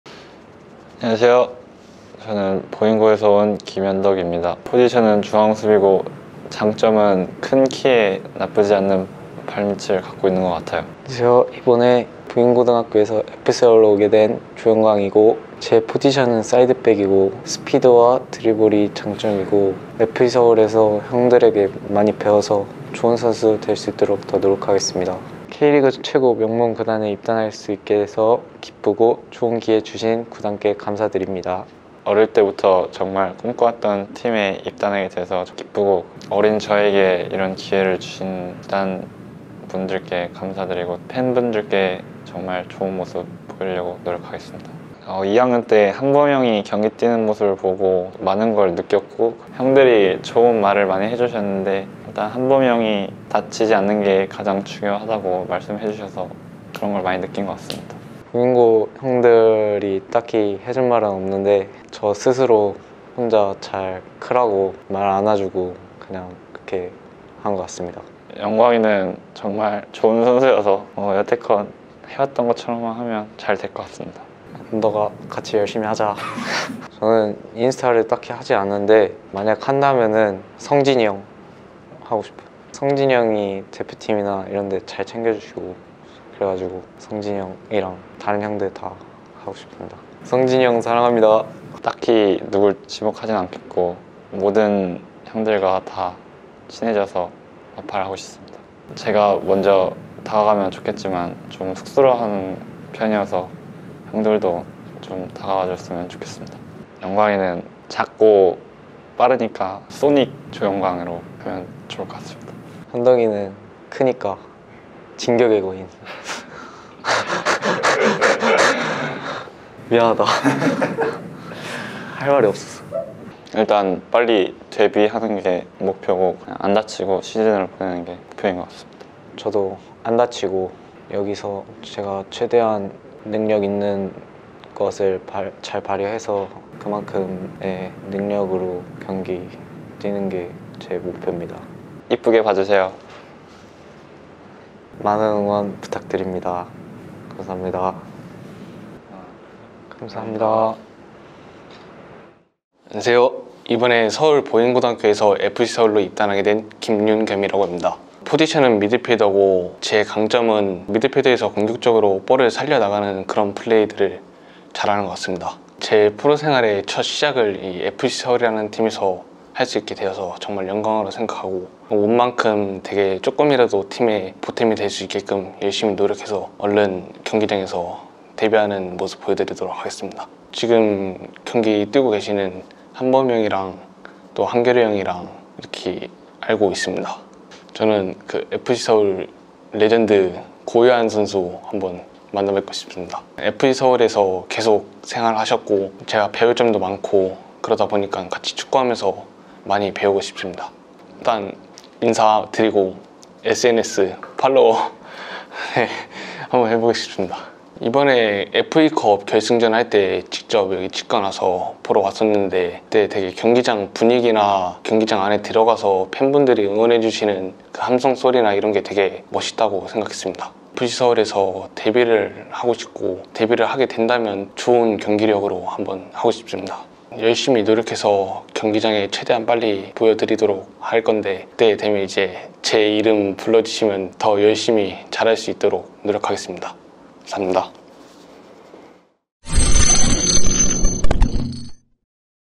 자유선발 신인 조영광-김현덕-김윤겸의 입단 인터뷰
FC서울이 신인 자유선발 자원으로
조영광, 김현덕, 김윤겸 선수와
프로 계약을 맺었습니다.